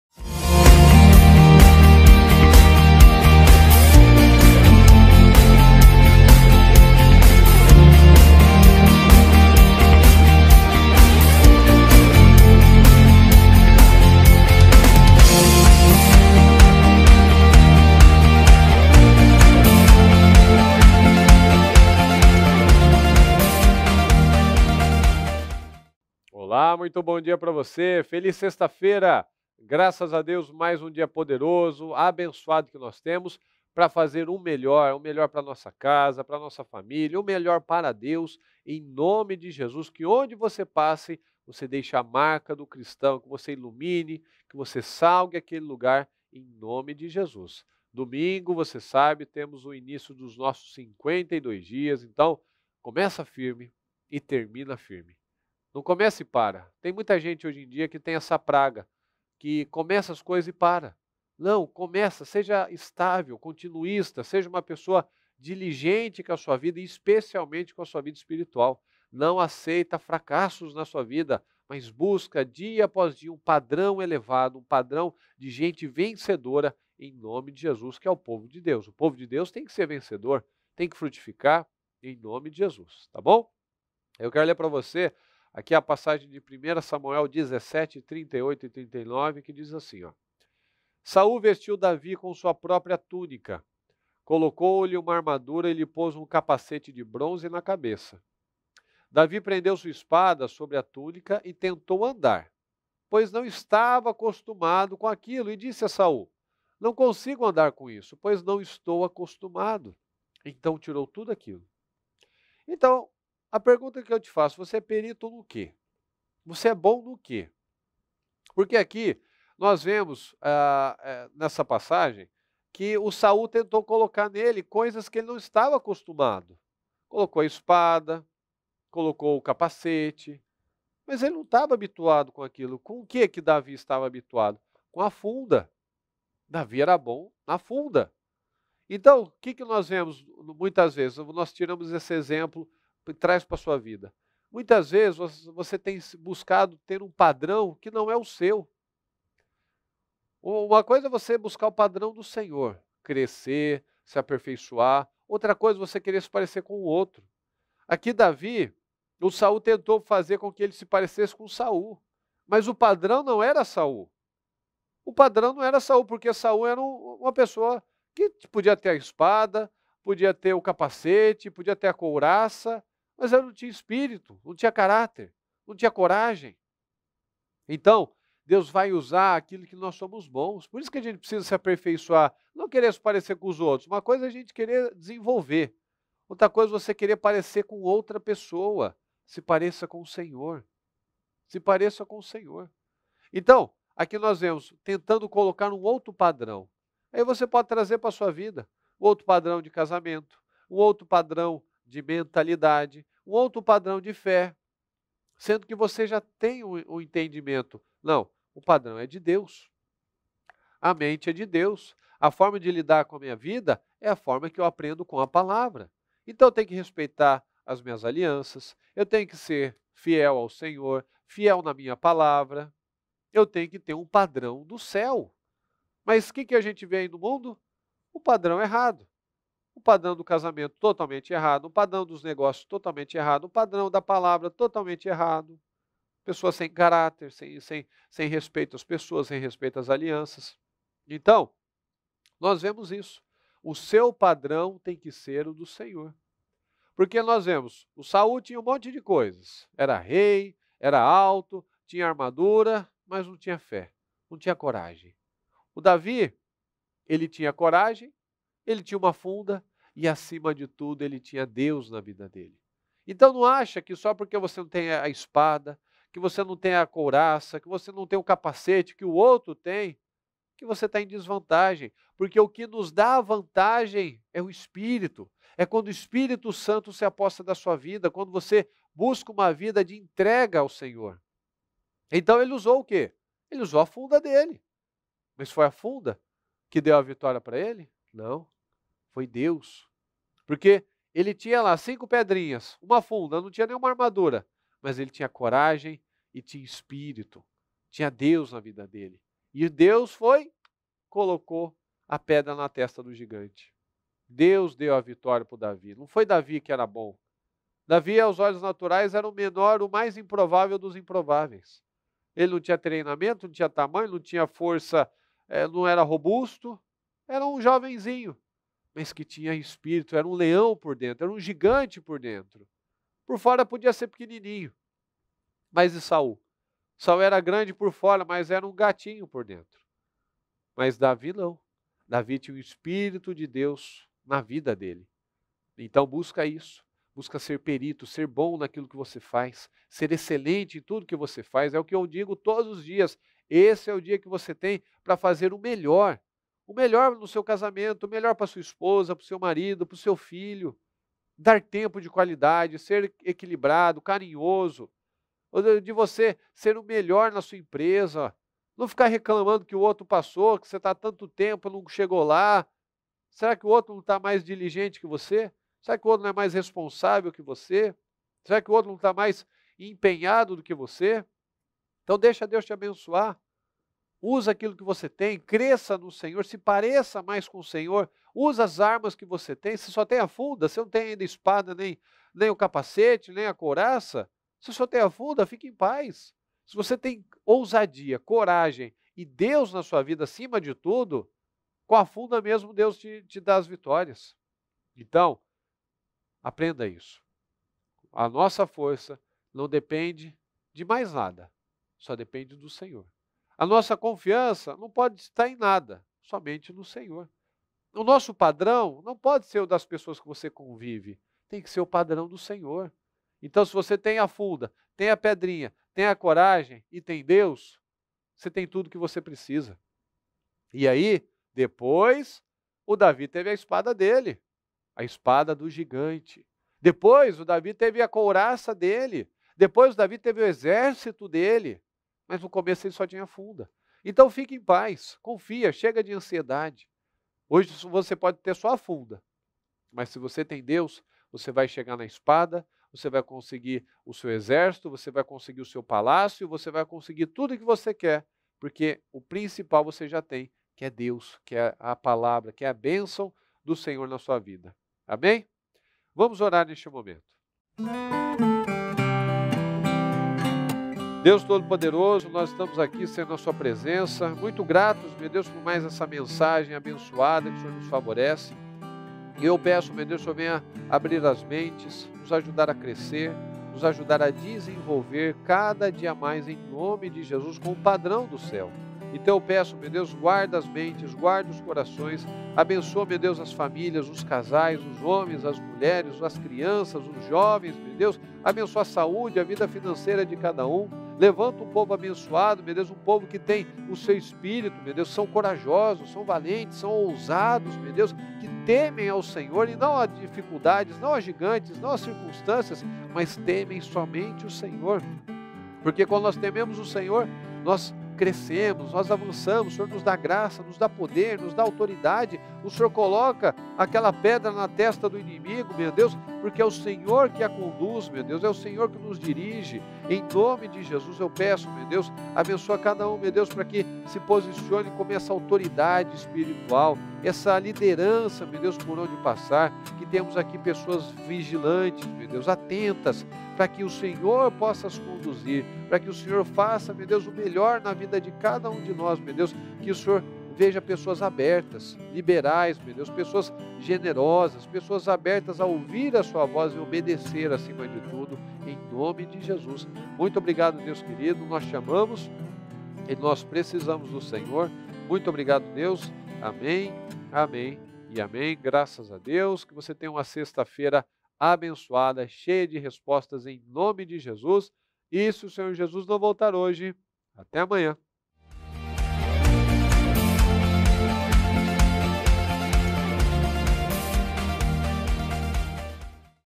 É perito em que? | Pensamentos de Bem
Data: 02 de julho de 2021

Você é perito em que?
Muitas vezes podemos estar buscando padrões que não servem para as nossa vidas, por isso é necessário ouvir sempre a voz do Senhor e identificar o padrão D'Ele para nós. Decida hoje não viver tentando imitar, copiar algo ou alguém, mas sim copiar o padrão de Cristo!

Ref. 1 Samuel 17.38-39 (NVI)